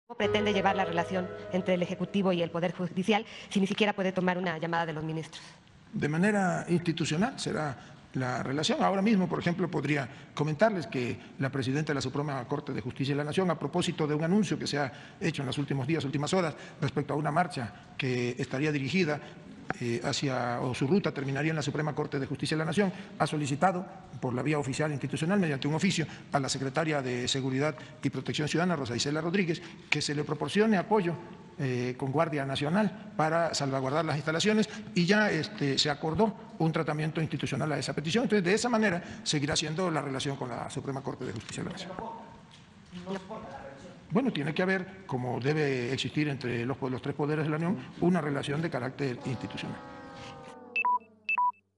Asegura Adán Augusto, que la relación con la SCJN será de manera institucional
Asegura Adán Augusto, que la relación con la SCJN será de manera institucional a pesar de que AMLO® le instruyó al gabinete no responderles ni el teléfono.